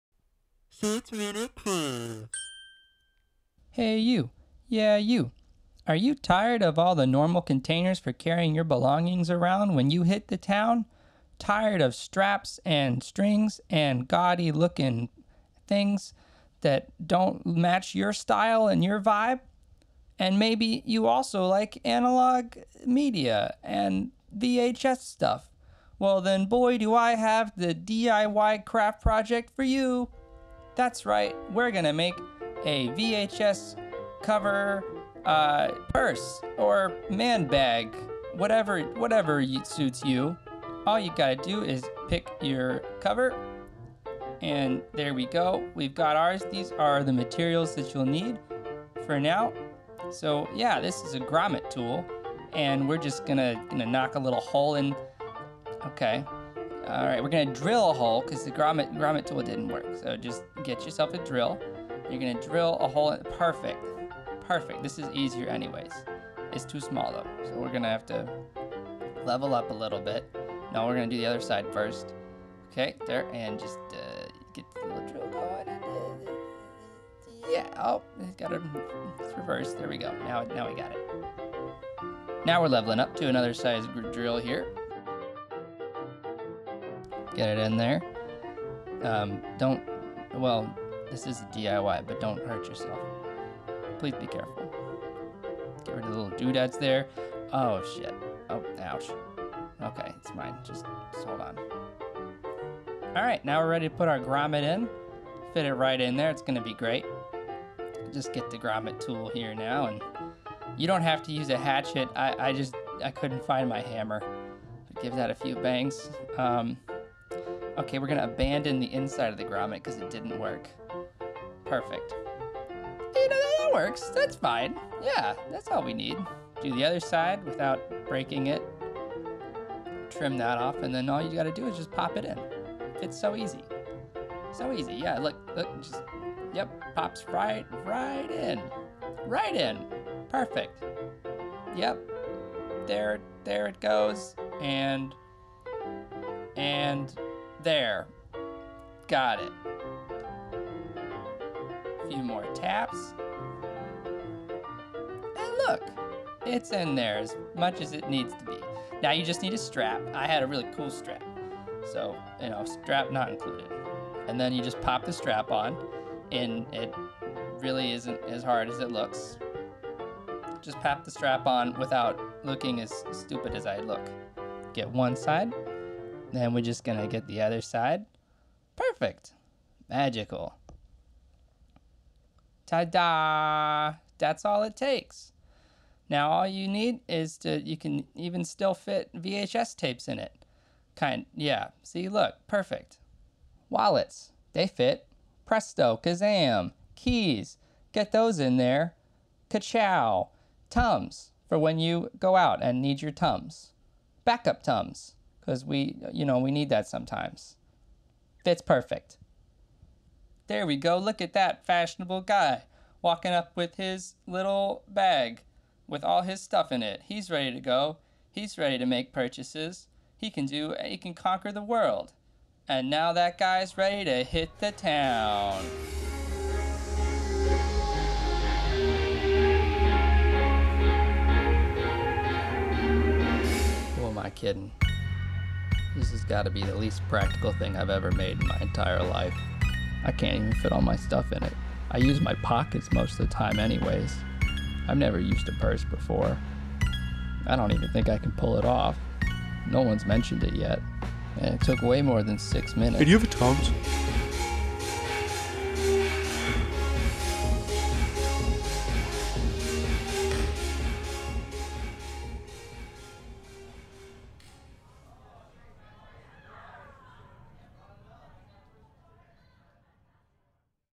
Make your own VHS clutch bag in 6 minutes
Now you can be the coolest cat when you hit the town sporting your very own DIY VHS purse manbag thing. Make your own craft in 6 minutes! Only for the hippest of hipsters.

0:00 - Intro
0:30 - Materials
0:49 - Making Holes
1:40 - Grommets
2:38 - Strap
3:05 - Getting Ready
3:46 - Hitting the Town